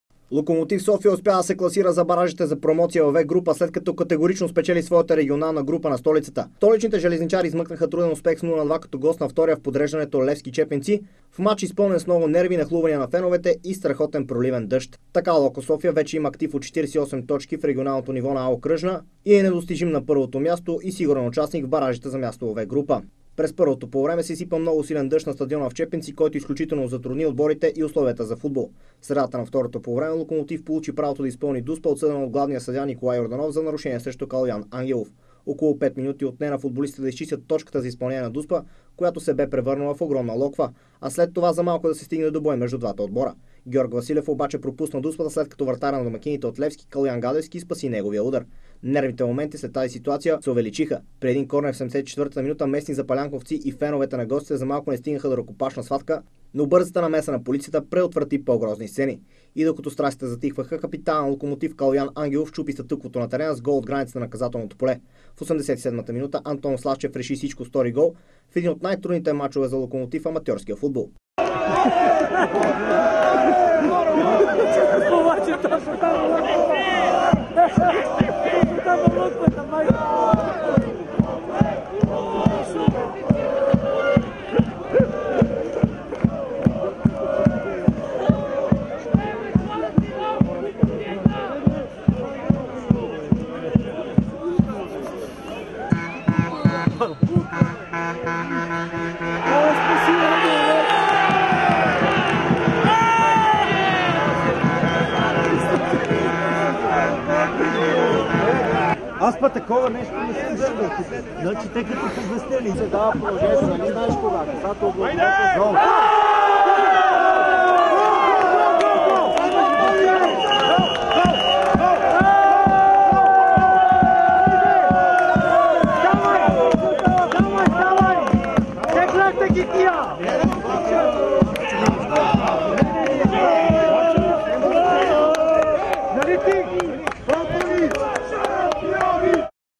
Локо София спечели първата част от ''А'' окръжна
Локомотив София успя да се класира за баражите за промоция във "В" група след като категорично спечели своята регионална група на столицата. Столичните железничари измъкнаха труден успех с 0:2 като гост на втория в подреждането Левски Чепинци в мач изпълнен с много нерви, нахлувания на феновете и страхотен проливен дъжд.